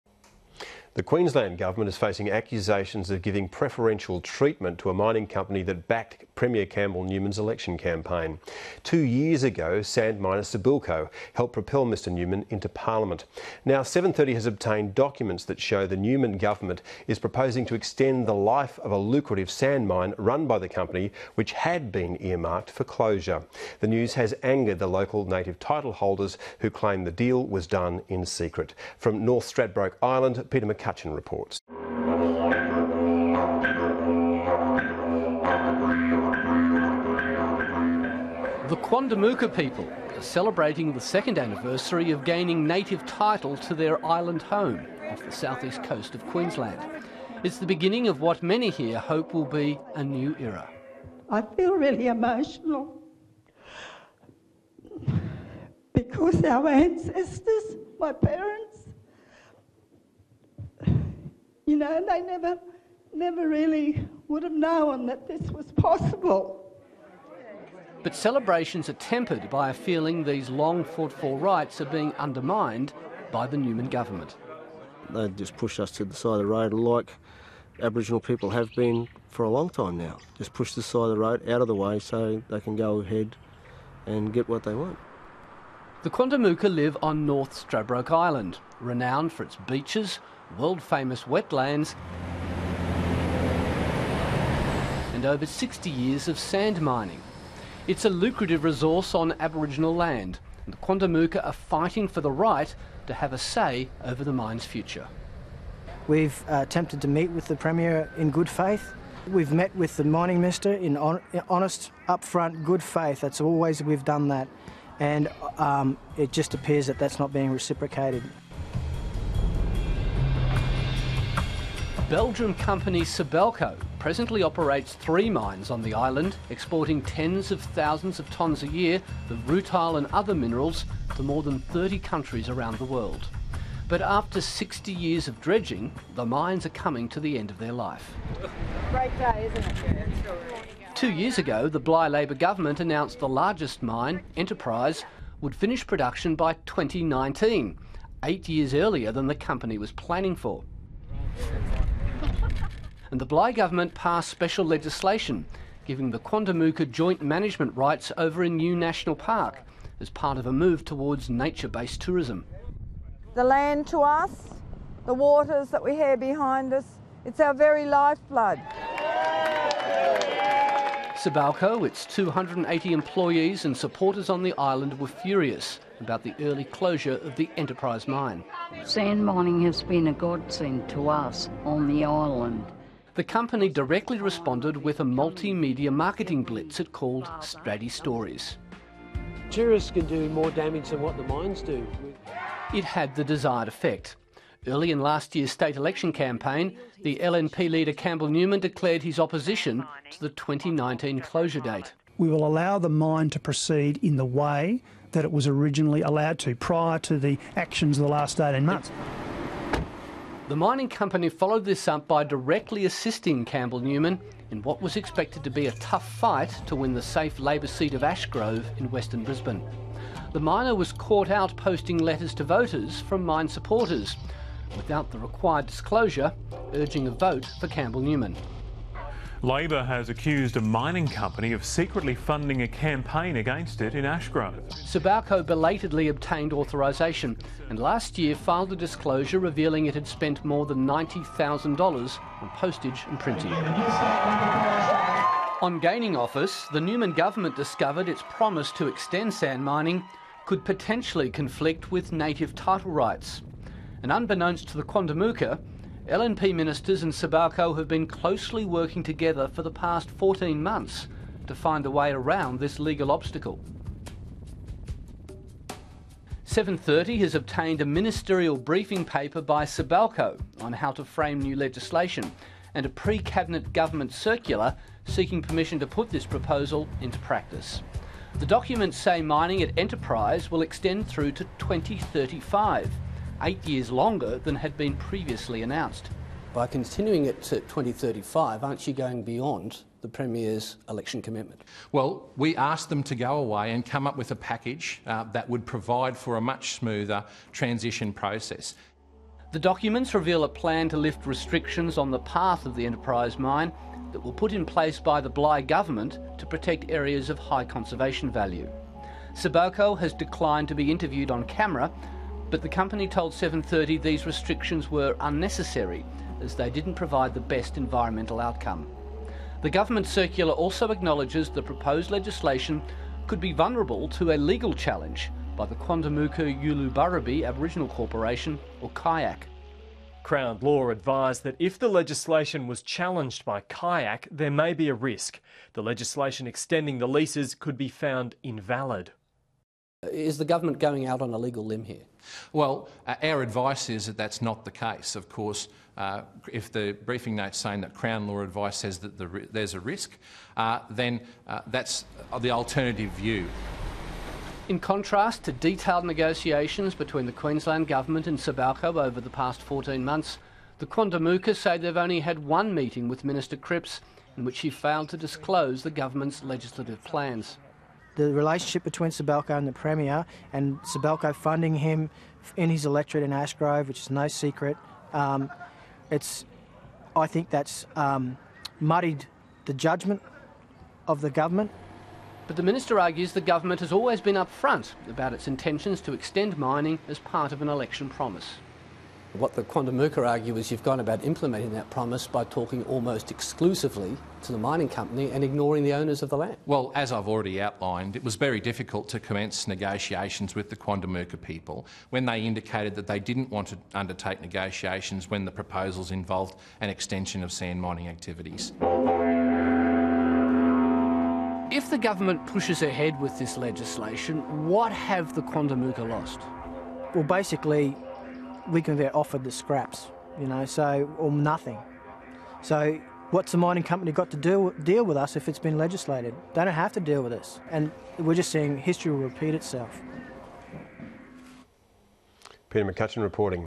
Quandamooka ignored: Nth Stradbroke sand mining granted to Campbell Newman's donor Sibelco
Quandamooka ignored: North Stradbroke sand mining extended to Campbell Newman's election donor Sibelco